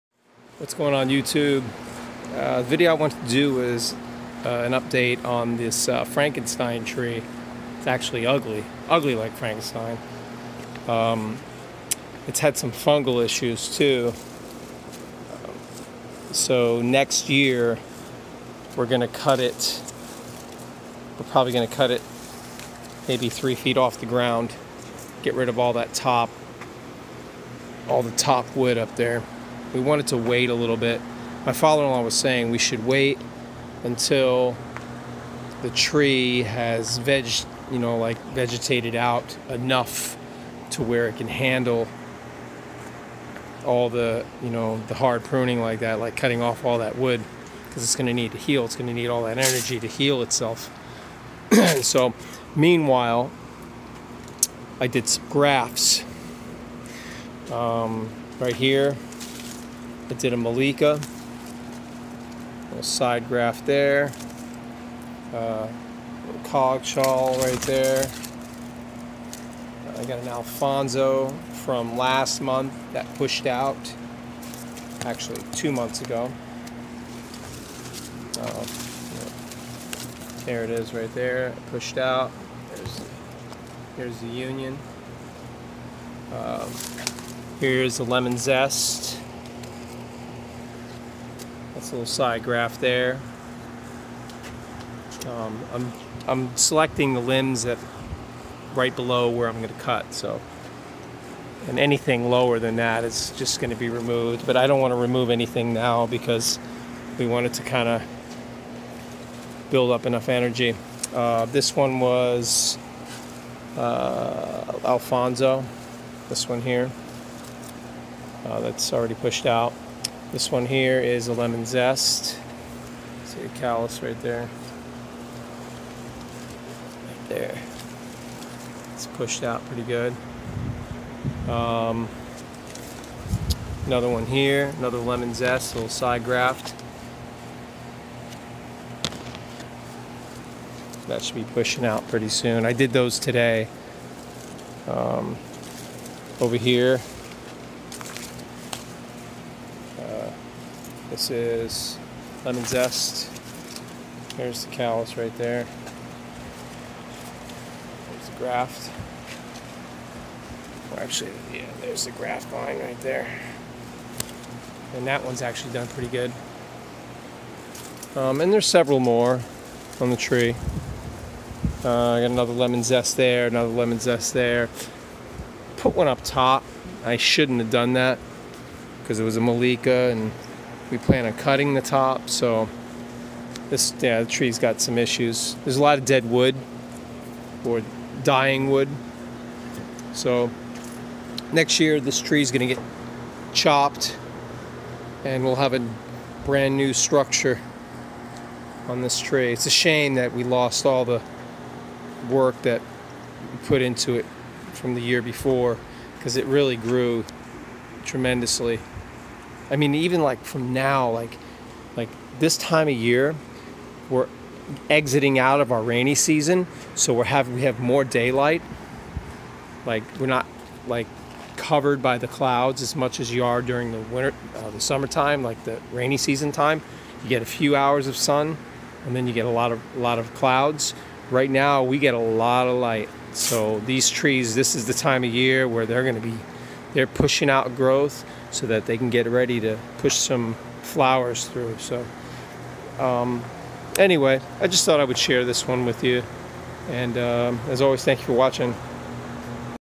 Grafting mangos🥭Multiple variety mango tree grafting project Frankenstein tree.
This tree suffered this past winter from a few cold nights.  All the progress I had made with the grafts was lost.  So I'm starting over.  Although I'm waiting to remove the remaining damaged wood until next year.